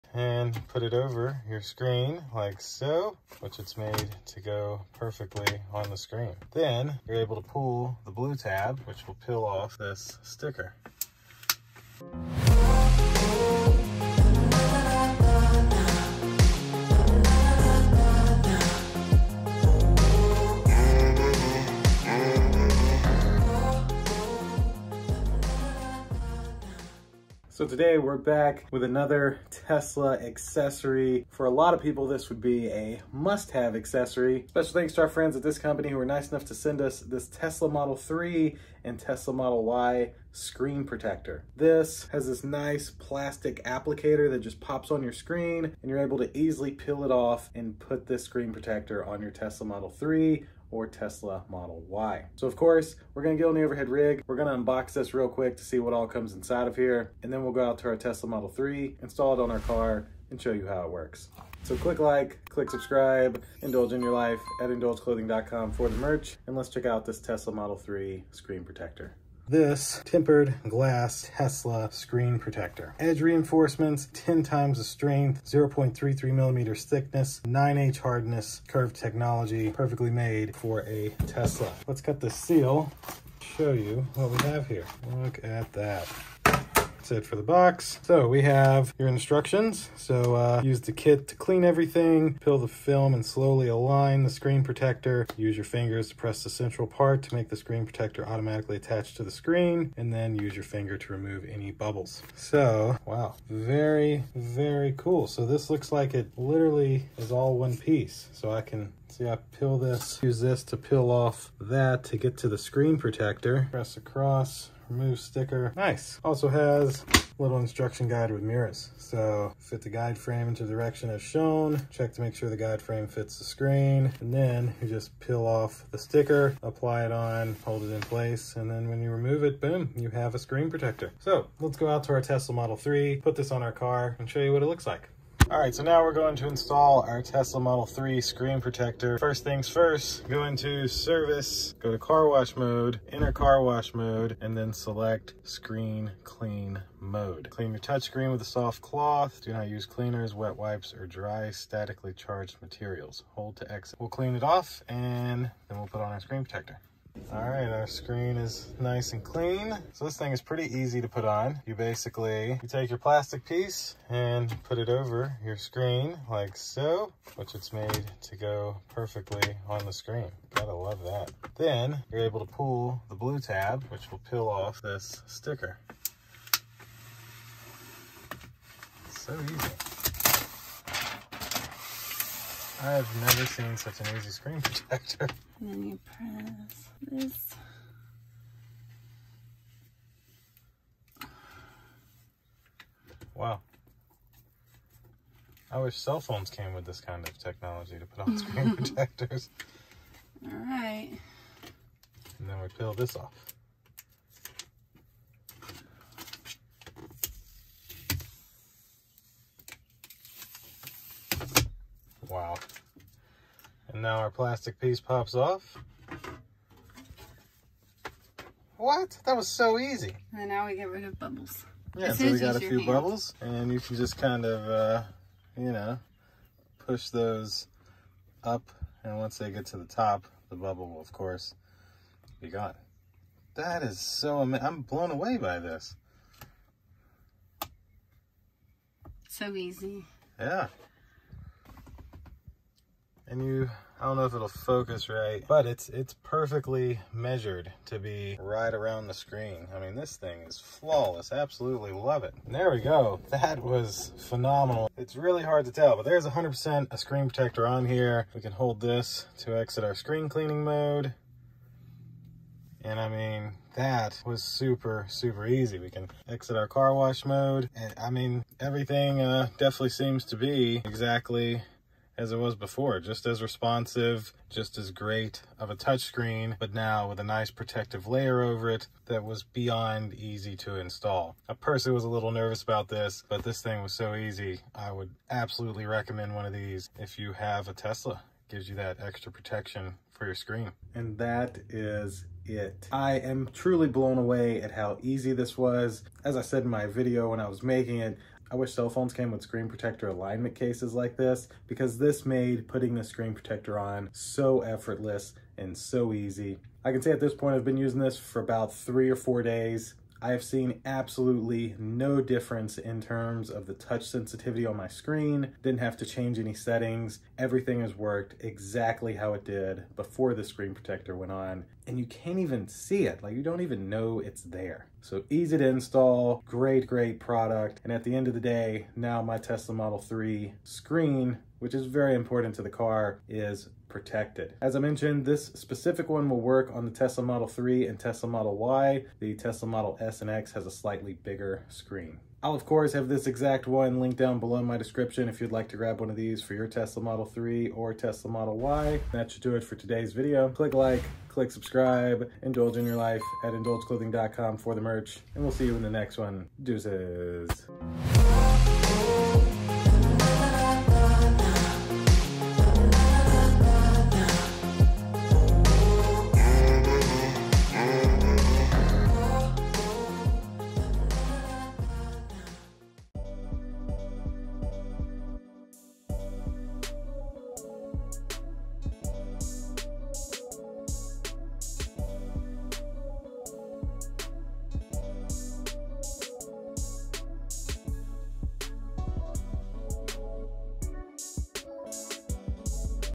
FUGTBLESS Screen Protector for Tesla Model 3 / Y Install & Review
Today we install and review the FUGTBLESS Screen Protector for Tesla Model 3 & Model Y! The install was beyond easy, and you cant tell a difference on your screen once this is installed. Check it out!

Do you want a FUGTBLESS Screen Protector for your Tesla Model 3 or Model Y?